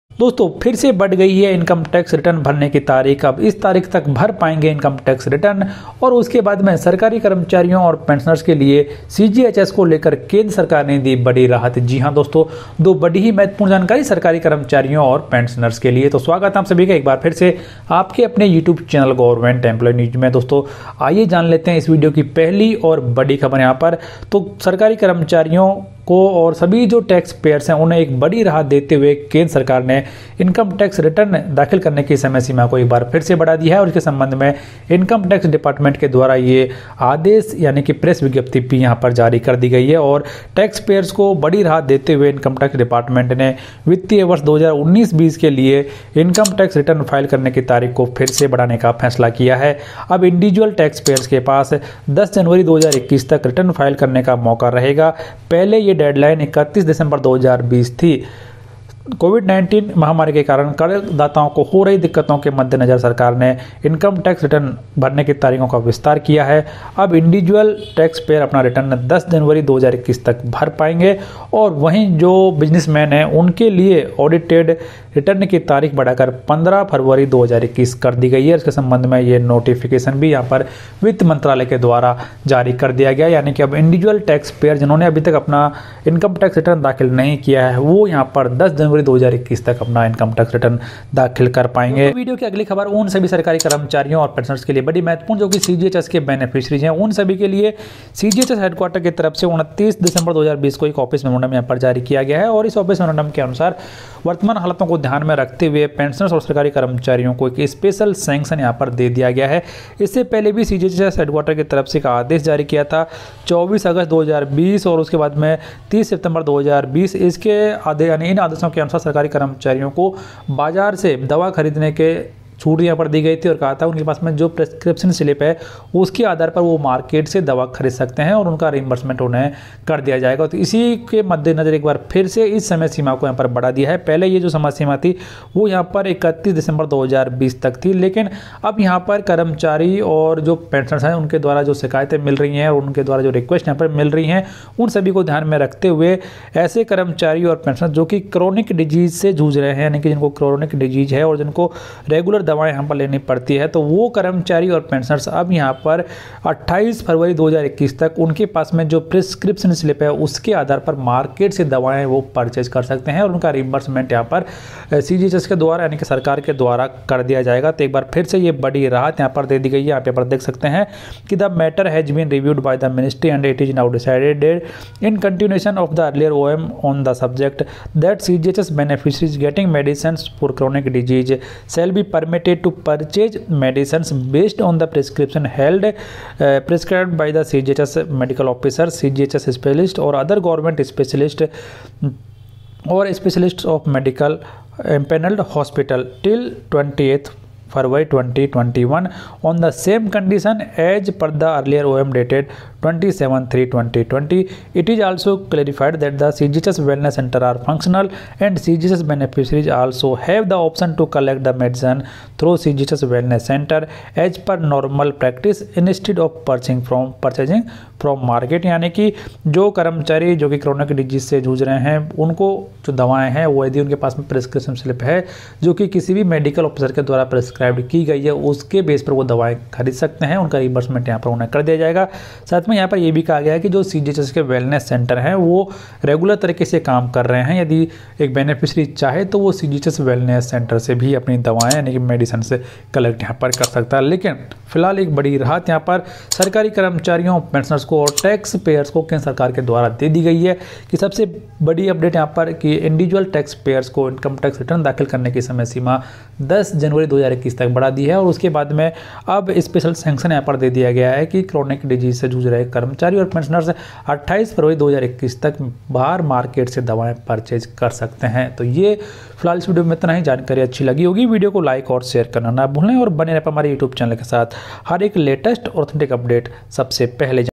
ITR 2020 Income Tax Last Date Extended, CGHS Special Sanction कर्मचारियों & पेंशनर्स के लिए
Today 2 Good News For Employee and Pensioners:
1. टैक्सपेयर्स को राहत देते हुए इनकम टैक्स डिपार्टमेंट ने वित्त वर्ष 2019-20 के लिए रिटर्न फाइल करने की तारीख को फिर से बढ़ाने का फैसला किया है. अब इंडिविजुअल टैक्सपेयर्स के पास 10 जनवरी 2021 तक रिटर्न फाइल करने का मौका है. पहले की डेडलाइन 31 दिसंबर 2020 तक थी.  ITR – Income Tax Returns filing deadline extended from Dec 31 to January 10, 2021 for these taxpayers COVID-19.
2. Special Sanction for CGHS card holders. OM regarding Reimbursement of OPD Medicines in view of COVID-19 extended upto 28 Feb 2021.